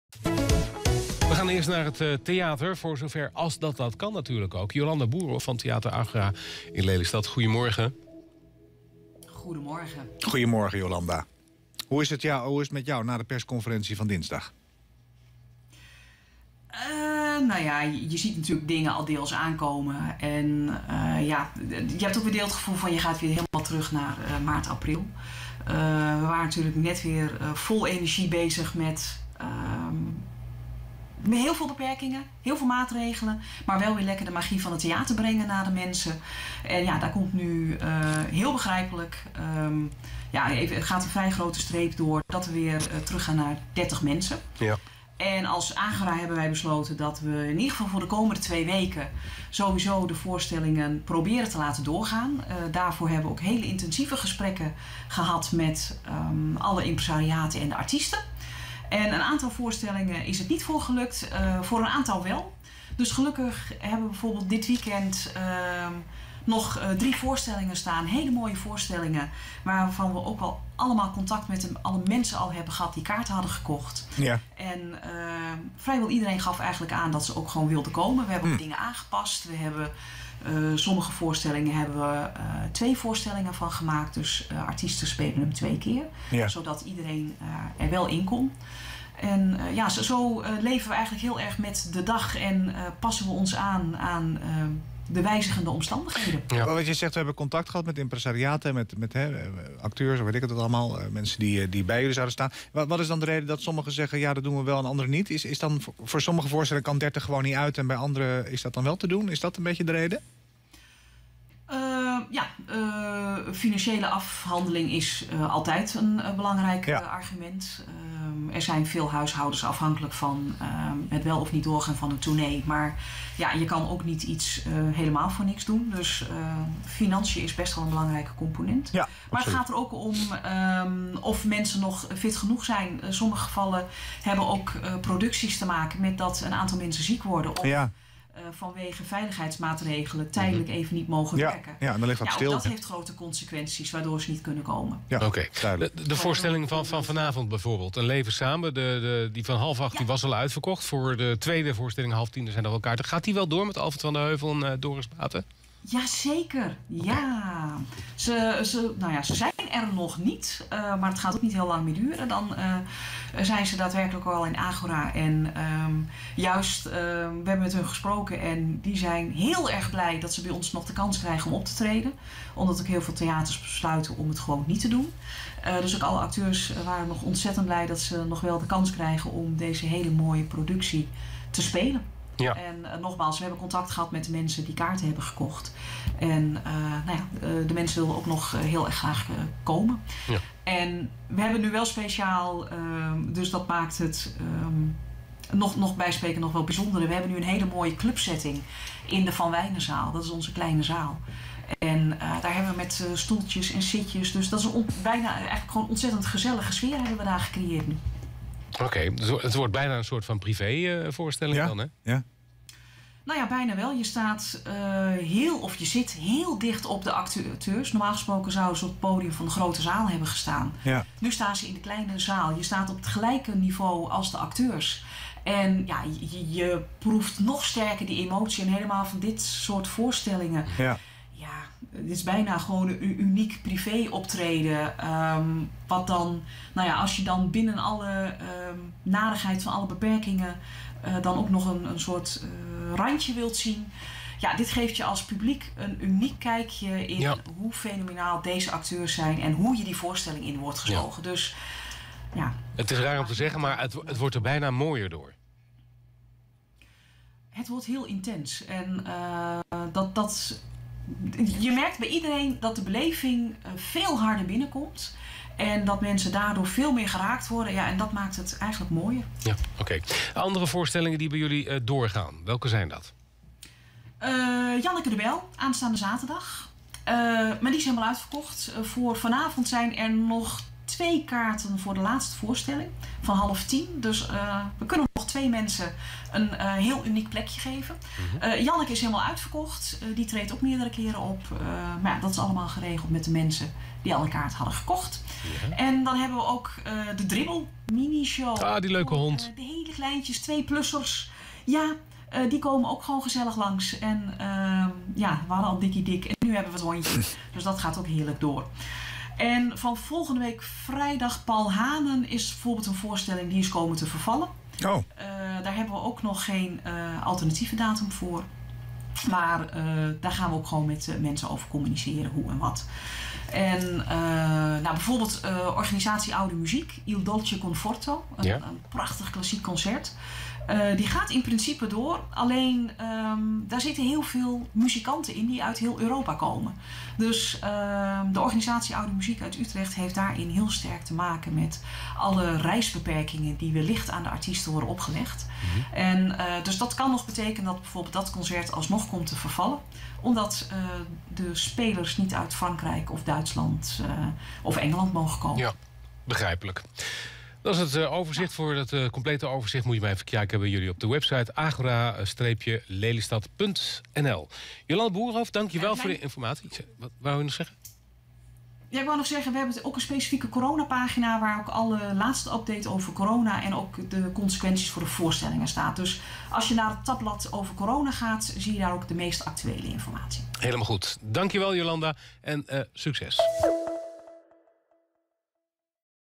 Binnenkort in het Agora
Wat is er de komende week te zien op de planken van het Agora Theater in Lelystad? Je hoort het van Jolanda Boerhof.